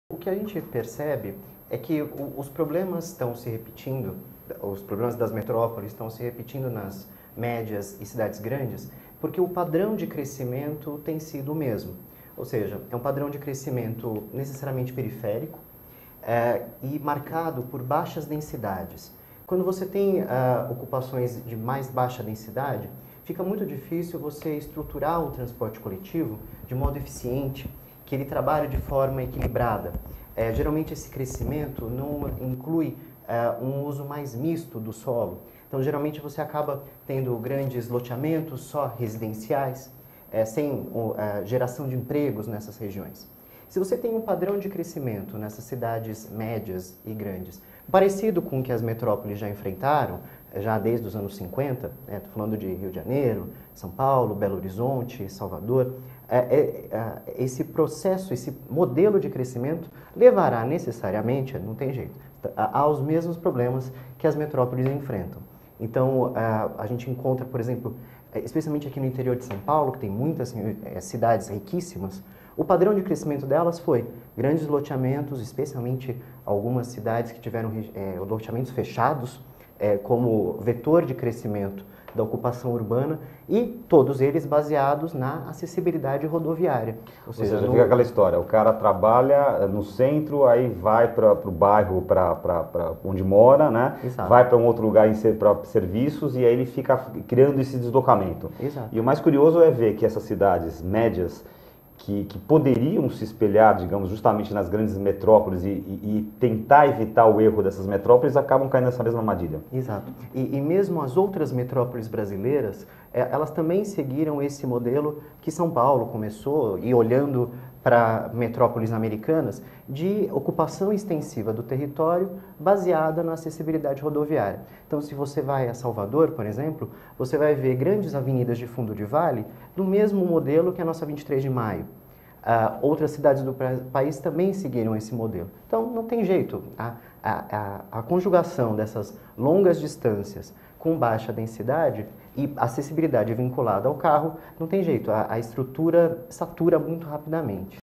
Mauricio Feijó - Minuto Urban View 1 - 02/07/2014
Mauricio Feijó fala sobre o desenvolvimento e crescimento padrão das cidades brasileiras.